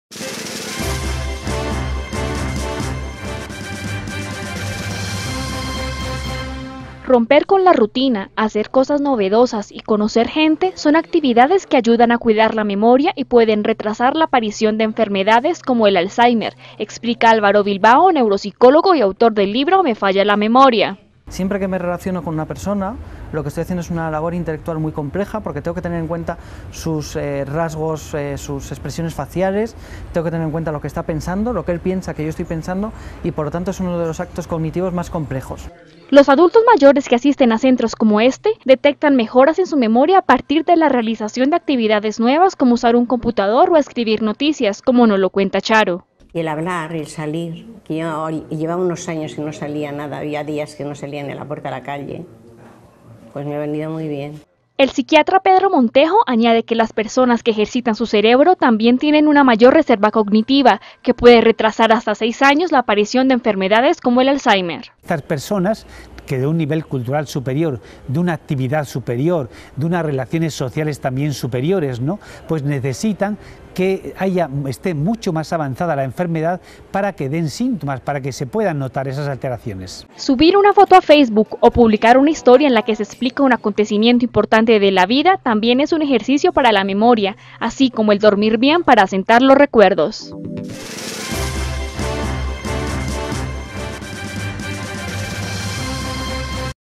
Estimular la memoria retrasa la aparición de enfermedades cerebrales
Para cuidar la memoria y retrasar la aparición de algunas enfermedades cerebrales es fundamental romper con la rutina, hacer cosas novedosas y conocer gente, explica Álvaro Bilbao, neuropsicólogo del Instituto de Mayores y Servicios Sociales (Inmerso), de España, y autor del libro "Me falla la memoria".

De acuerdo con el experto, el relacionarse con una persona es uno de los actos cognitivos más complejos "porque tengo que tener en cuenta sus rasgos, sus expresiones faciales, lo que está pensando y lo que él piensa que yo estoy pensando".
Los adultos mayores que asisten al centro Inmerso detectan mejoras en su memoria a partir de la realización de actividades nuevas como usar un ordenador o escribir noticias, así como mediante la interacción con nuevas personas.

Fuente: EFE